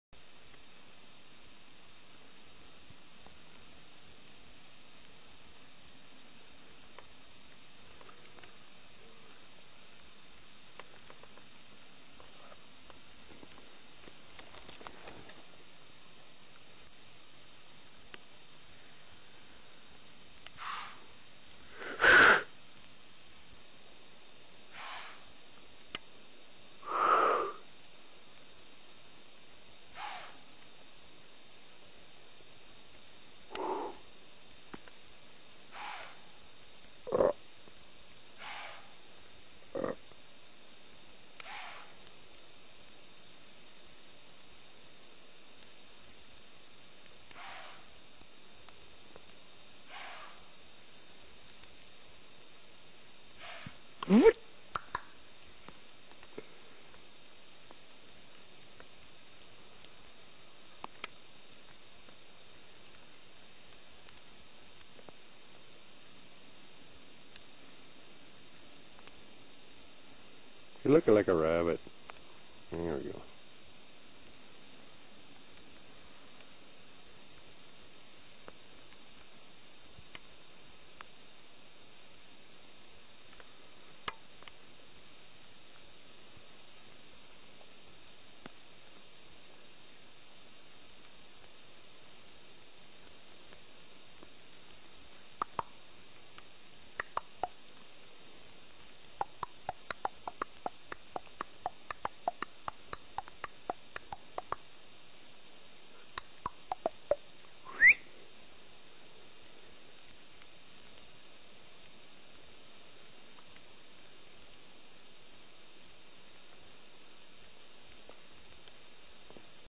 silly doe
playing with a doe i scared out of bed